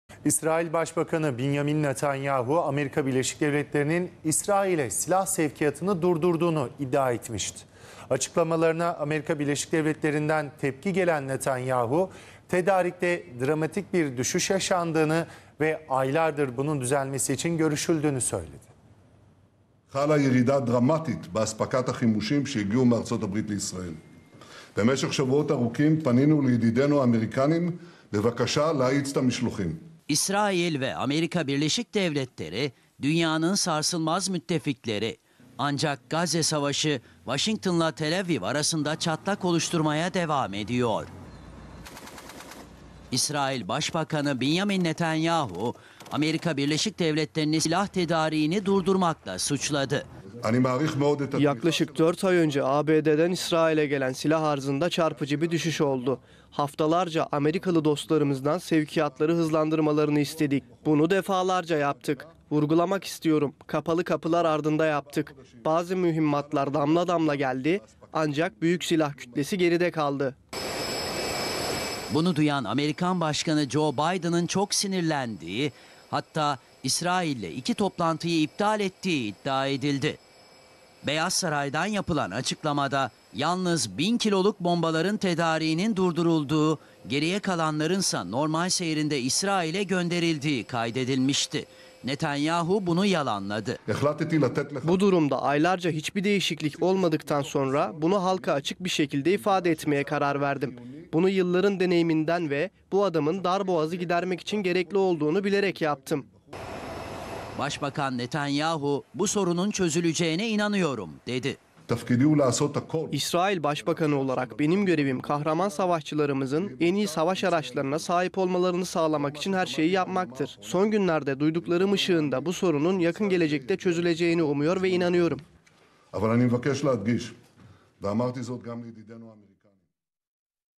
ABD'den İsrail'e Cevap! ABD İsrail'e Silah Sevkiyatını Durdurdu Mu?
İsrail Başbakanı Netanyahu, ABD'nin İsrail'e silah sevkiyatını durdurduğunu iddia etmişti. ABD'den o iddiaya yanıt geldi.

En son haberler, en güncel gelişmeler ve uzman yorumcuların haber analizleri CNN TÜRK ekranında canlı yayınlar ile izleyicisiyle buluşuyor.